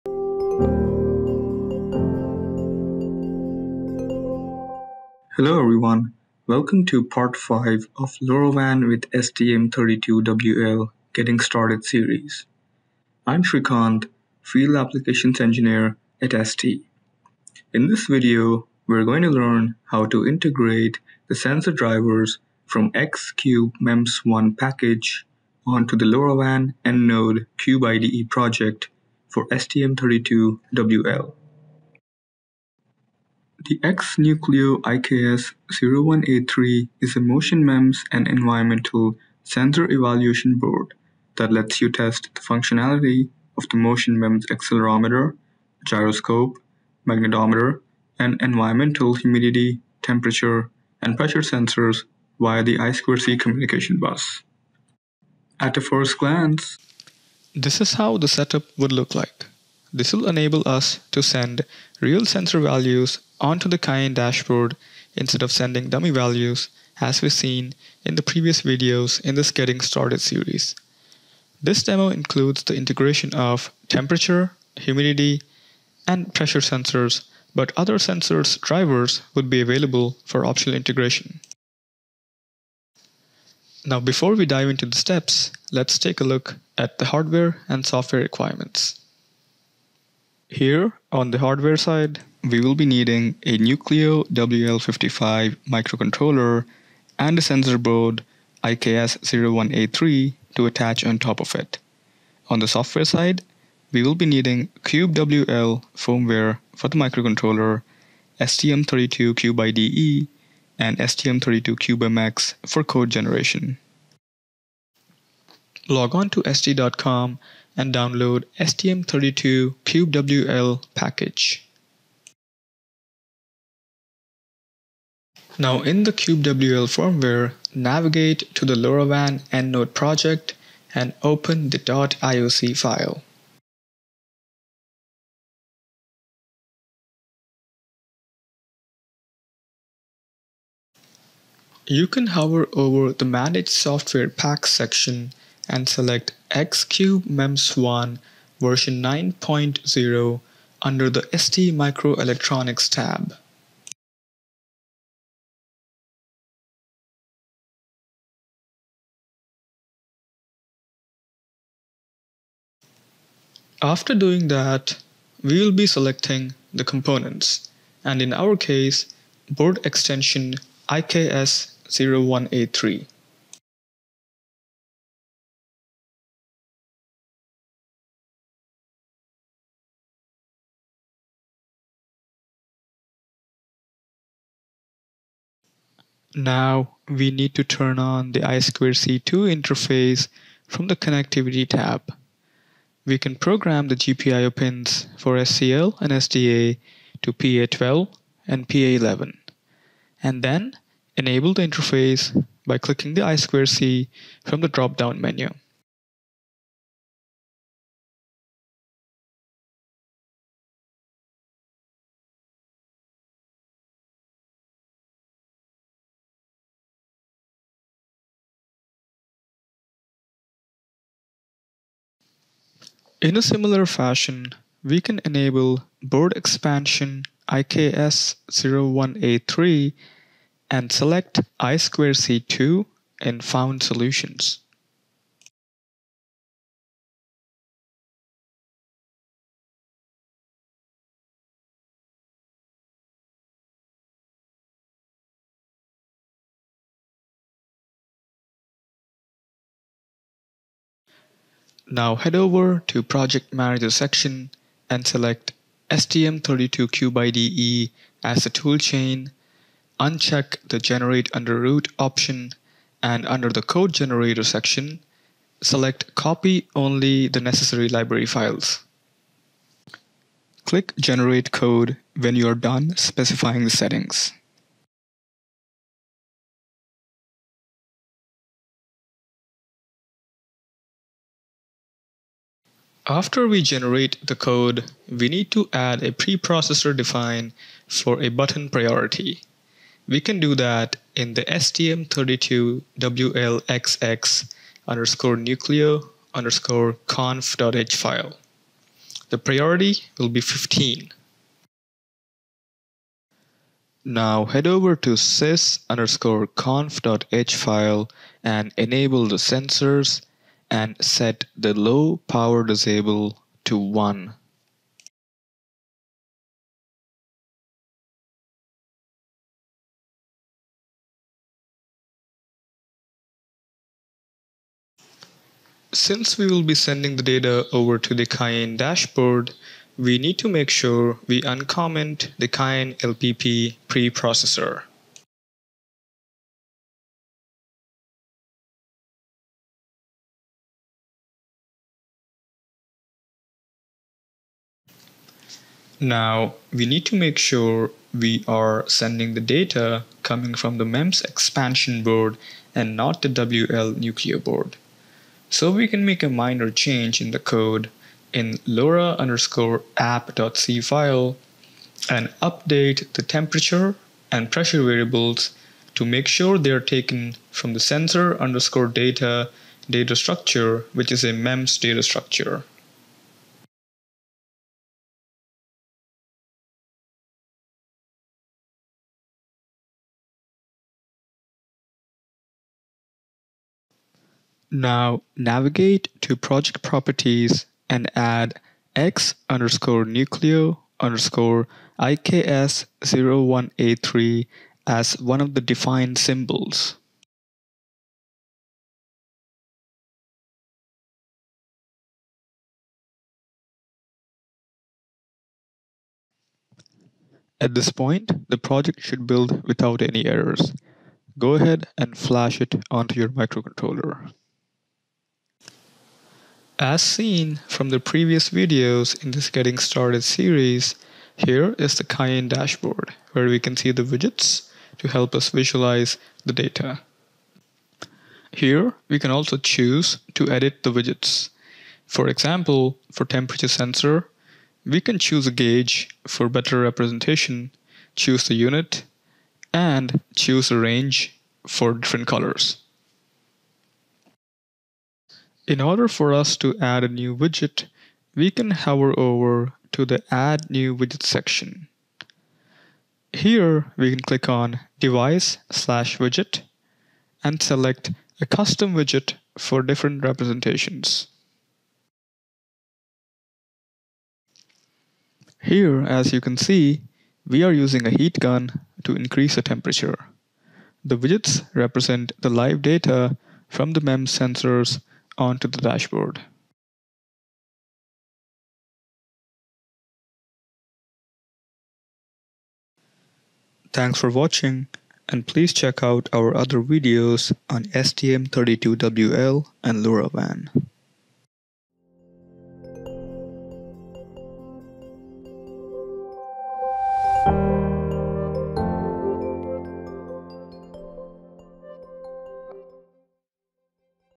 LoRaWAN with STM32 Getting Started: Part 5, IKS01A3 Sensor Shield Integration - STM32CubeIDE
This video is intended to demonstrate how to integrate some of the sensor Drivers from X-NUCLEO-IKS01A3 MEMS Sensor shield (X-Cube-MEMS Package) on to the NUCLEO-WL55JC, End Node project (STM32CubeWL v1.1) using STM32CubeMX.